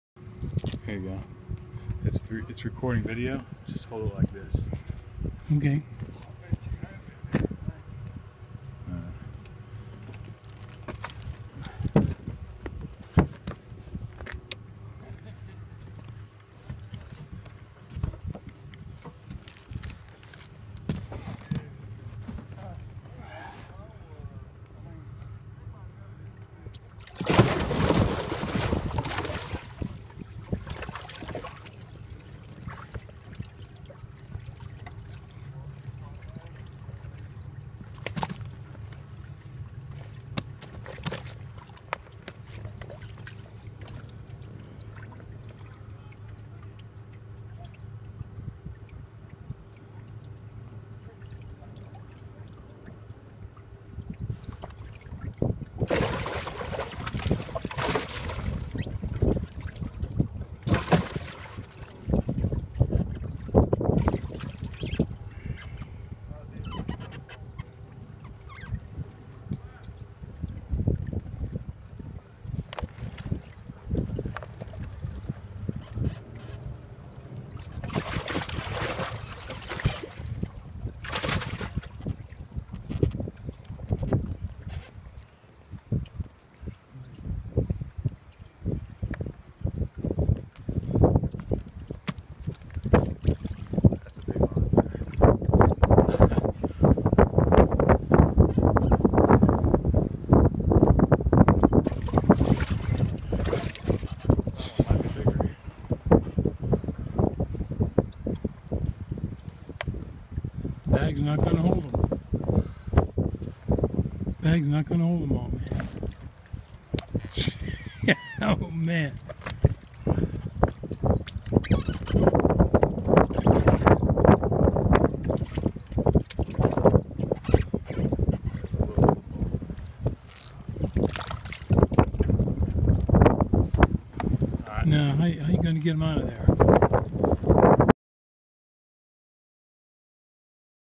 Sacking up 28.58 Jordan Lake NC Bass Tournament 16 April 2016
Got 1st place and big bass in a tournament on Jordan Lake NC on 16 April 2016. It was great that pops was there and got to be part of the fun.